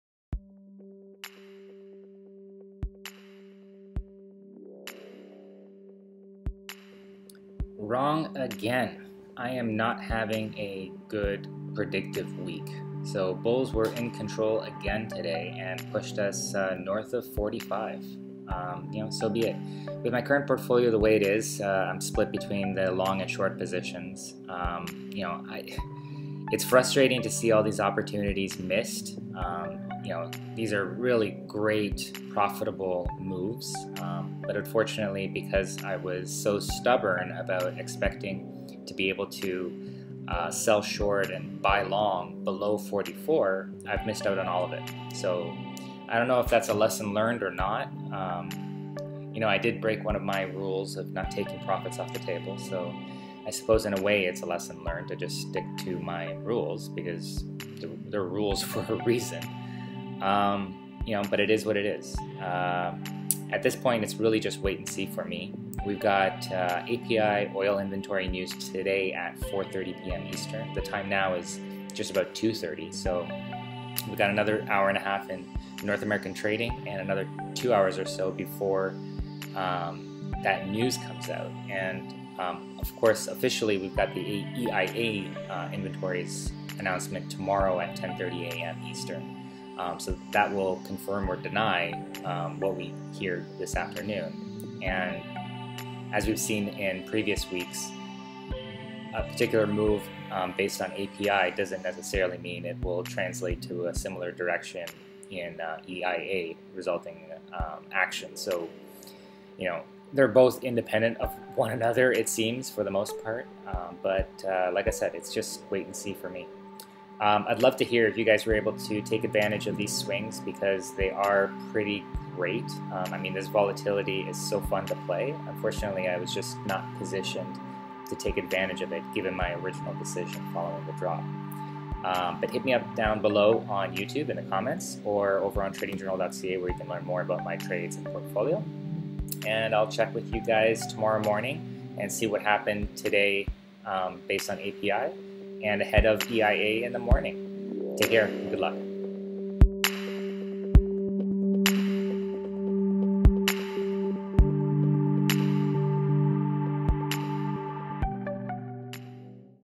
35 - Results for July 11, 2017 - Wrong again... bulls in control ahead of news
* Wrong again today… bulls seem to be …well, bullish
* With my current portfolio, there’s nothing for me to do, but wait
* API Oil Inventories news at 4:30p EST today so we’ll see what happens after hours
* EIA (Official) Inventory news tomorrow at 10:30a will confirm or deny what we get at 4:30 today
* For now… for me… it’s just wait and see